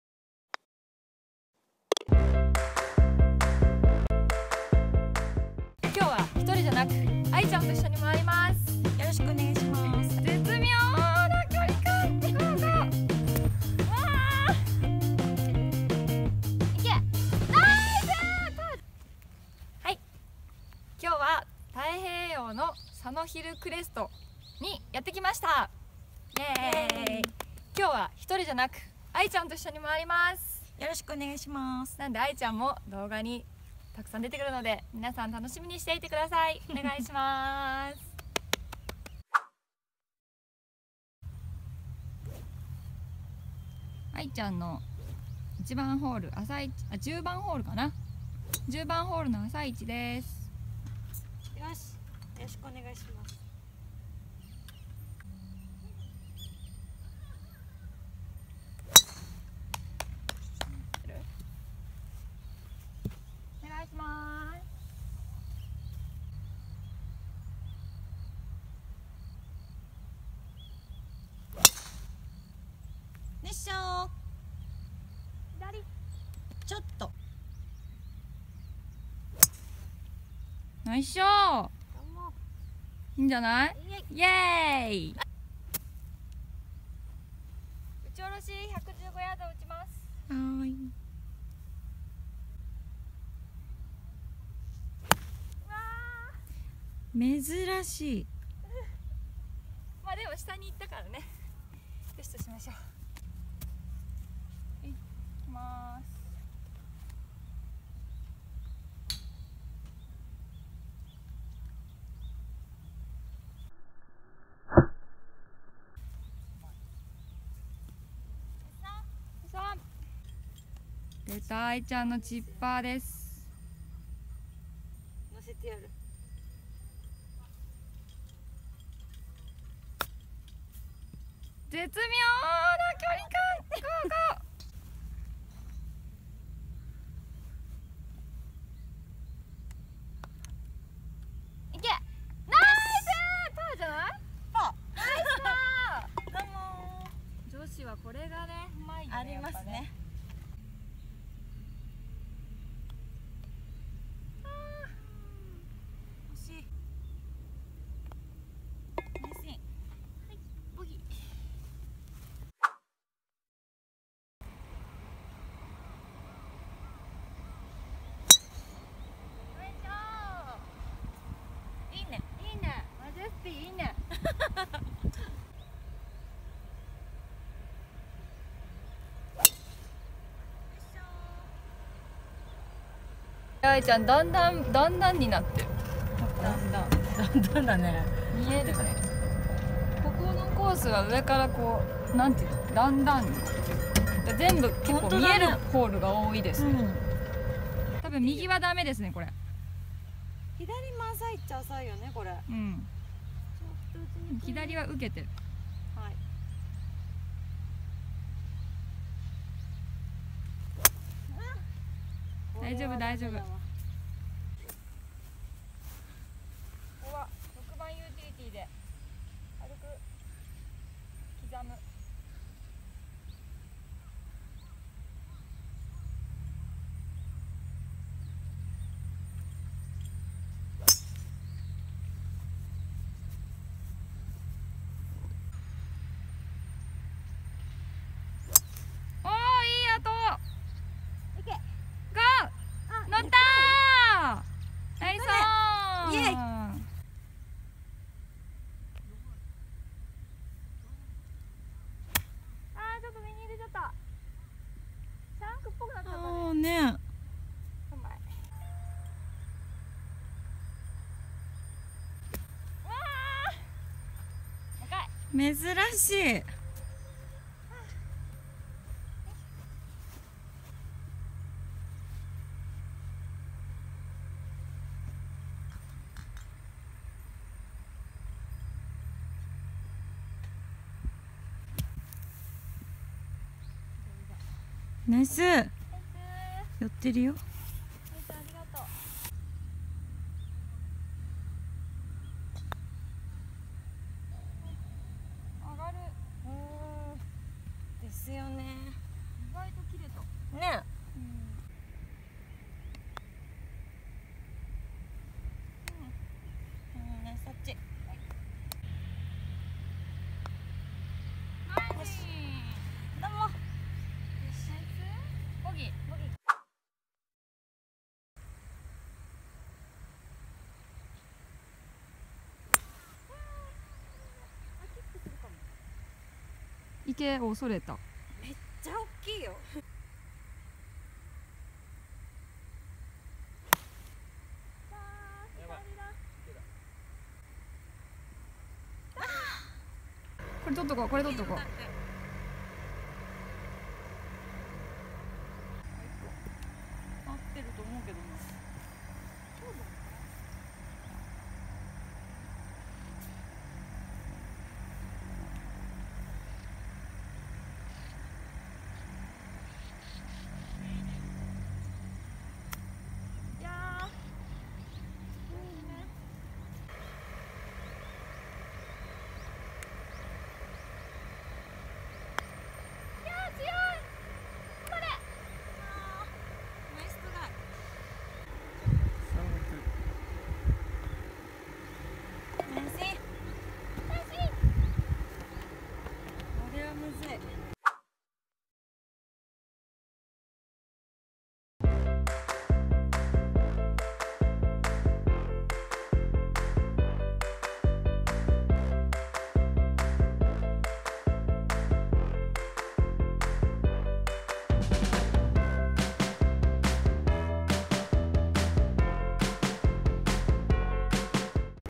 栃木で二人で女子ゴルフ！池ポチャもあります。【太平洋クラブ佐野ヒルクレスト】[1-3H] 三枝こころ&あい
♣今日の動画
今日のゴルフは栃木！太平洋クラブ佐野ヒルクレストにやってきました。友達のあいちゃんと一緒にレギュラーティをラウンドします。

1H 334yd PAR4
あいちゃんとはプライベートでもよく一緒にラウンドしているゴルフ友達です。二人でラウンドする動画は楽しさがさらに出てきますね。

2H 383yd PAR4
打ち下ろしのコース。あいちゃんの3打目の７Wが絶妙。
三枝こころは本日も苦手なバンカーに果敢に挑戦しています。
グリーンの微妙な傾斜を読むのが難しい〜。

3H 152yd PAR3
左に池のあるパー3、池を意識してゴルフをしていると、、、

♣本日のゴルフ場: 太平洋クラブ 佐野ヒルクレストコース